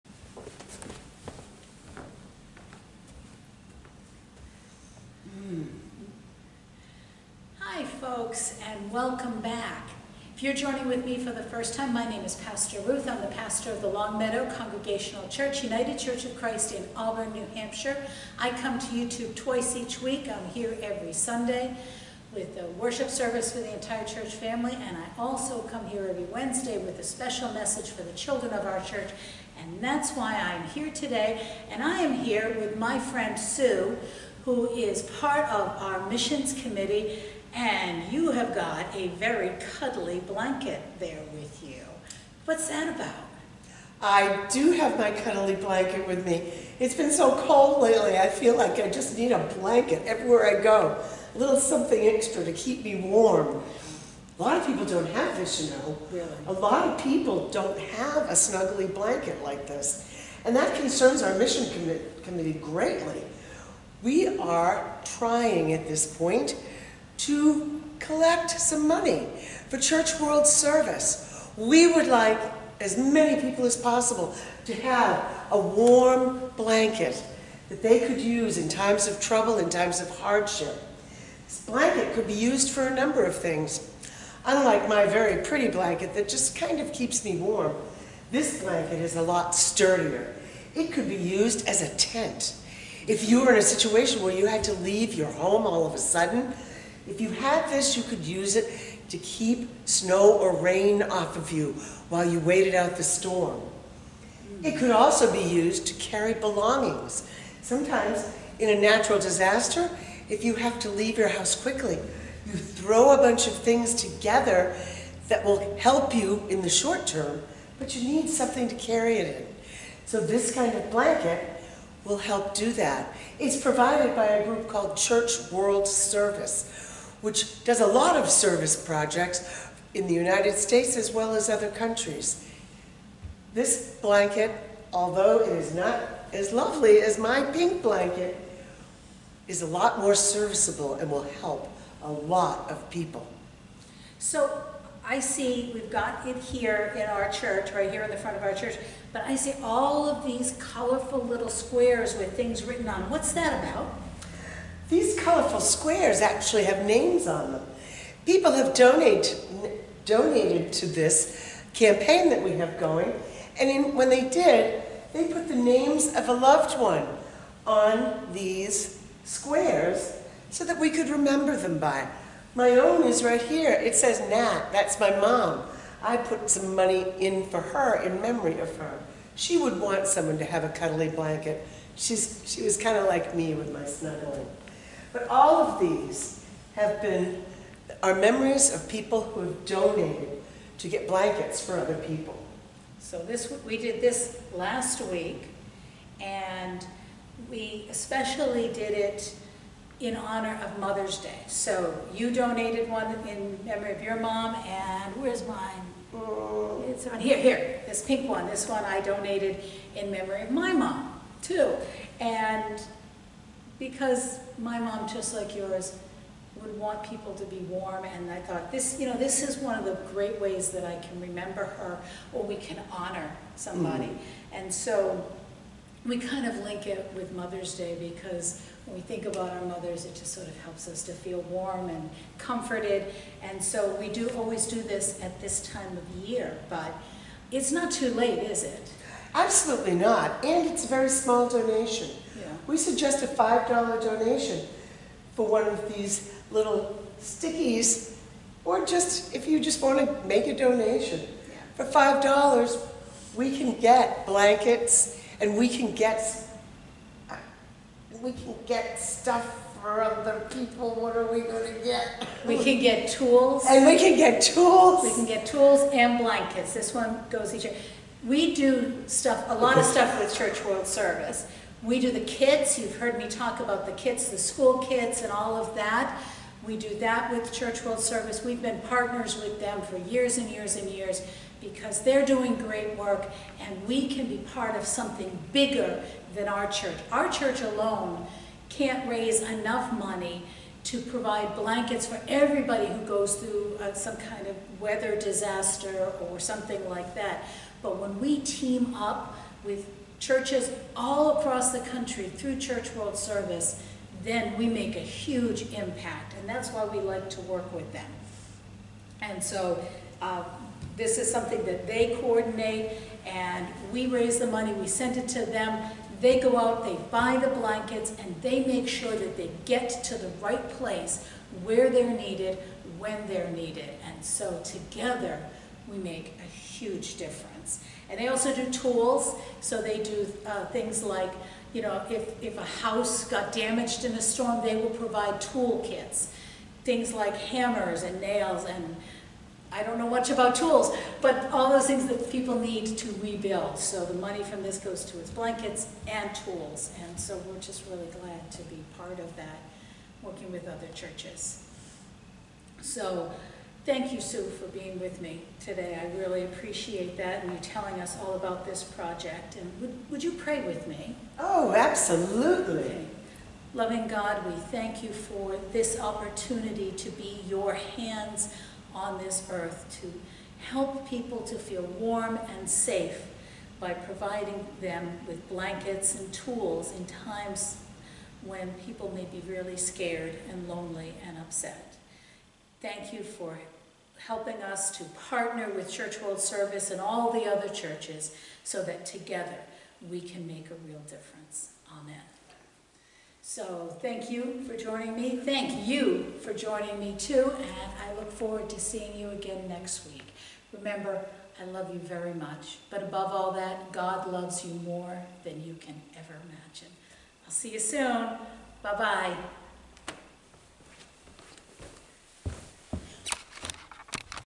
Children's Message 5/15/24 CWS Blankets +
Message for the children of the Longmeadow Congregational Church UCC of Auburn, NH on this third week of May.
My name is Pastor Ruth of the Longmeadow Church and I post a special message for the children of our church every Wednesday as well as on Sunday for Family Worship.
Today, I am joined by Sue who is a member of our Missions Committee to tell us more about our annual project to raise money and partner with Church World Service to buy blankets to help people in emergency situations.   Let’s talk about it…